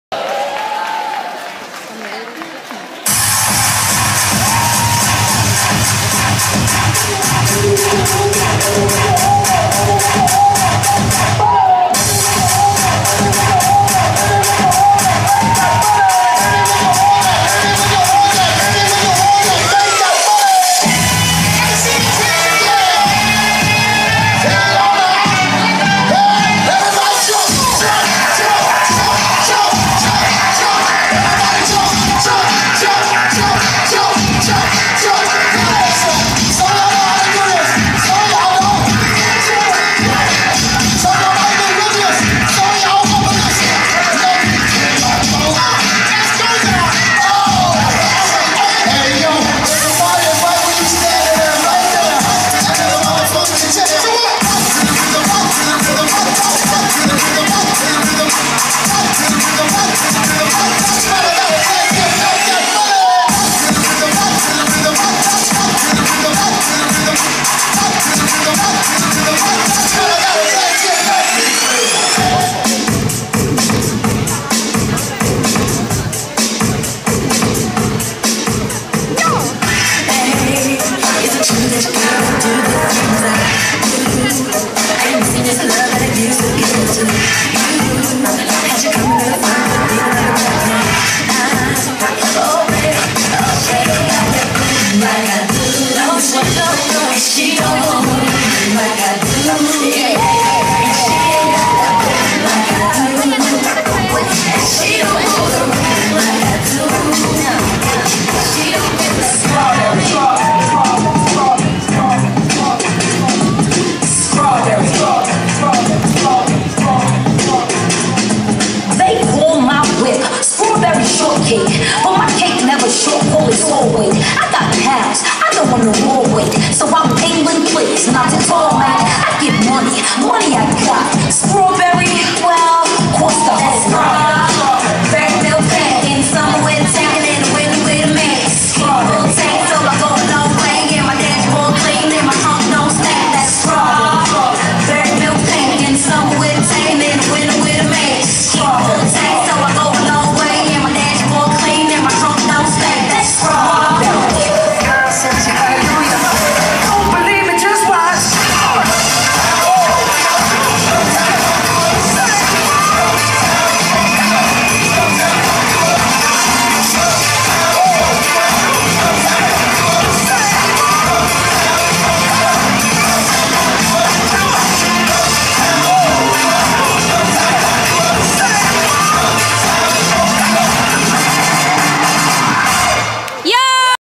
Stage juniors - CDO DLT 2015 Lužiny 19.4.
Soutěžní team Stage juniors na Pražském regionálním kole formací CDO Dance Life Tour 2015